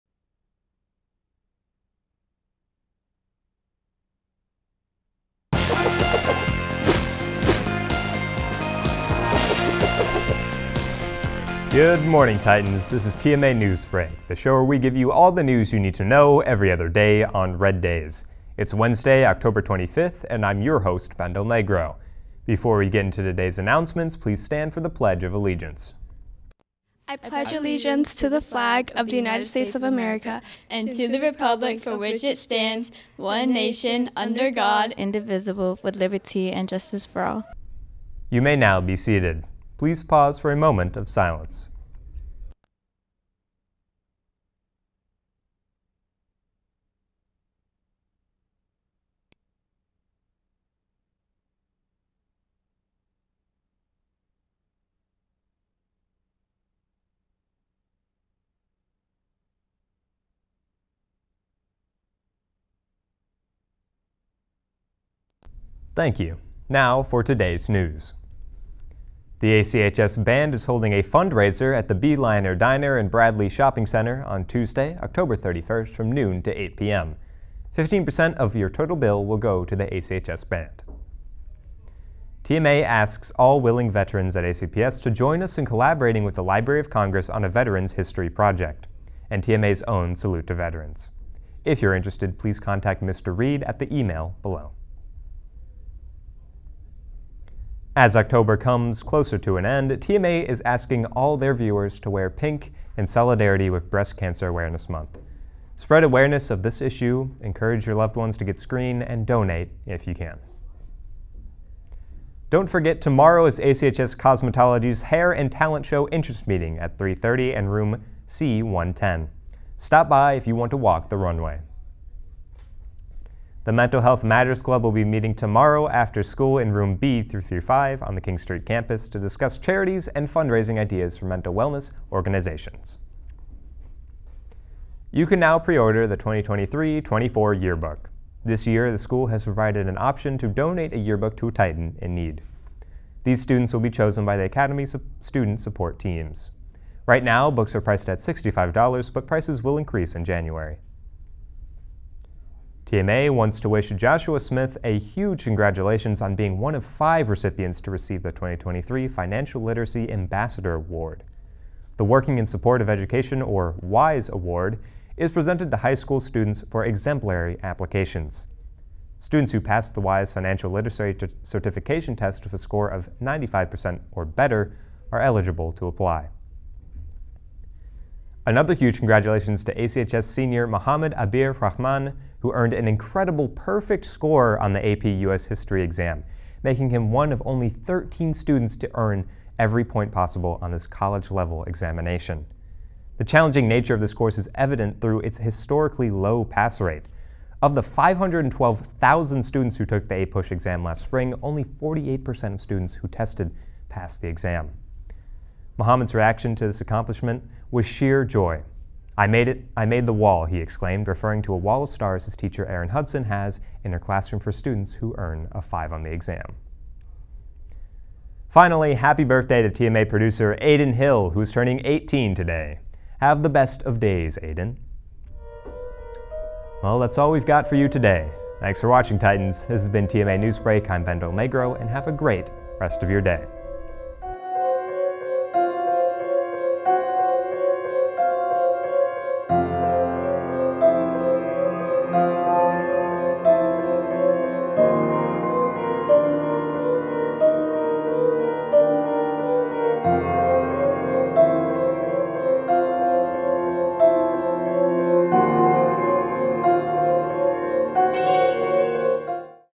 Titan News Break, October 25, 2023
Alexandria City High School's student run daily announcement/news show.

Director - Sarah McBurney
Technical Director - RJ Diaz-Paz
Audio Director - Rocky Hernandez
Graphics Creator - Amanda Houseman
Graphics Operator - Amanda Houseman
Assistant Director - Vieve Anderson
Prompter Operator - Stella Huff
Camera Operator - Aiden Hill
Host - Ben Delnegro